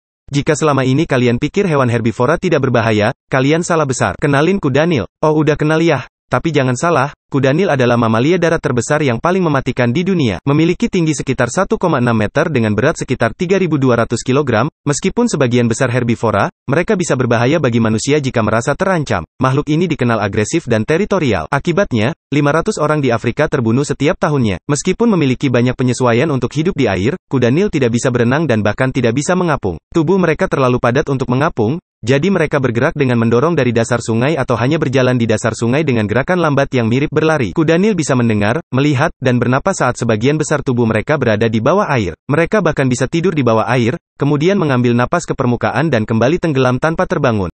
Herbivora Yang Ditakuti Buaya | Kuda Nil
Uniknya kuda Nil yang meskipun herbivora tapi berbahaya untuk manusia dan buaya.